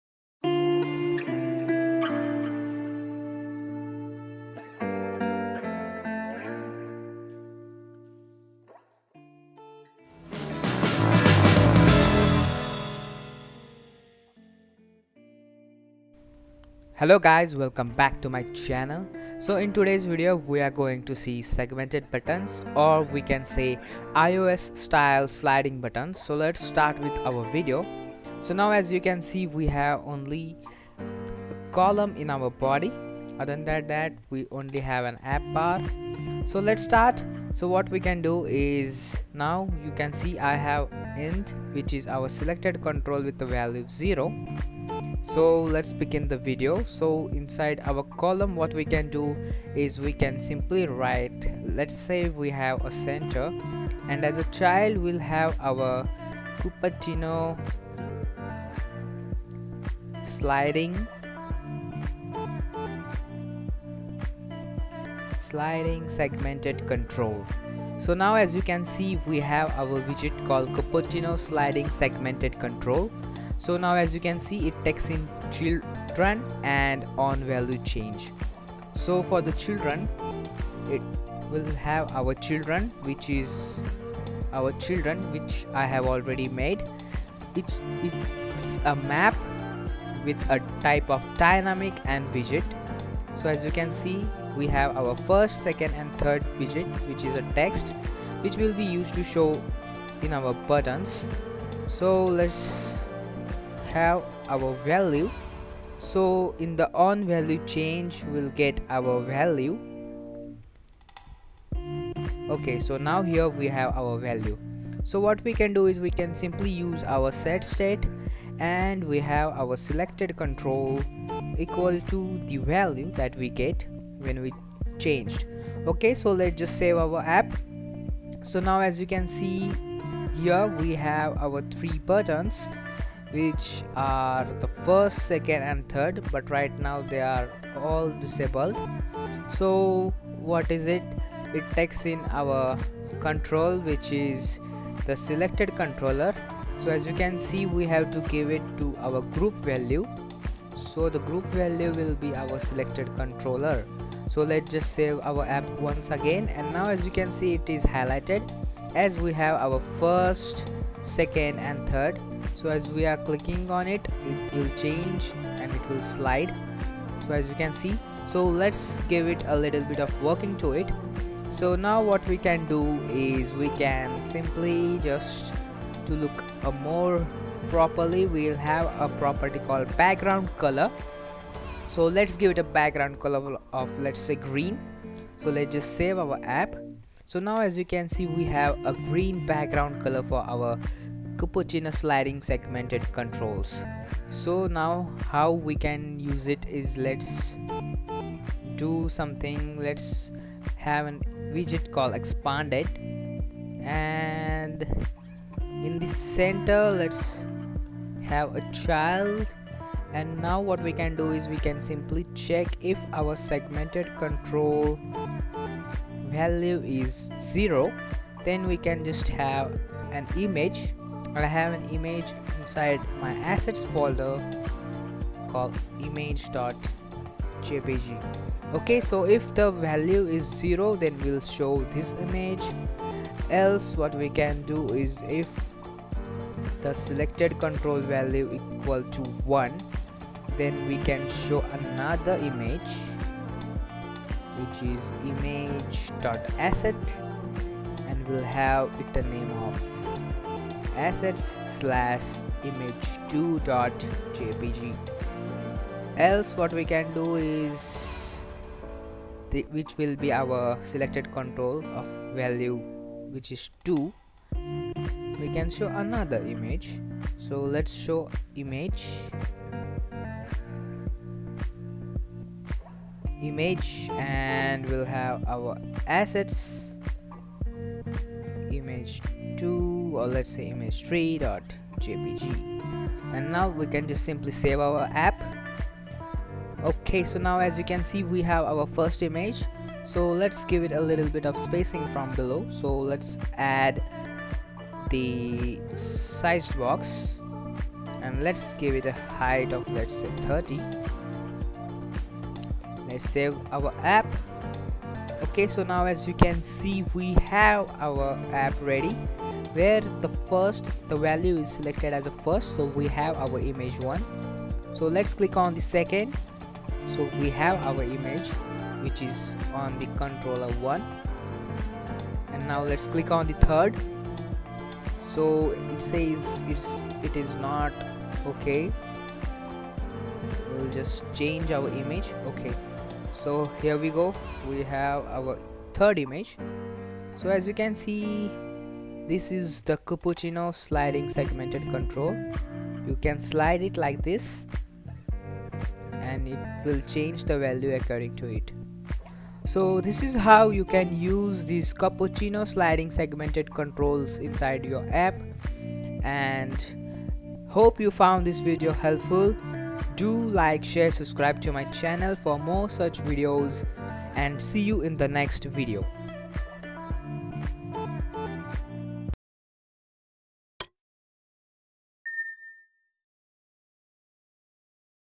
Flutter CupertinoSegmentedControl Widget Explained | Flutter Tutorials
In this Video learn how you can use CupertinoSegmentedControl widget in Flutter, if you found this video helpful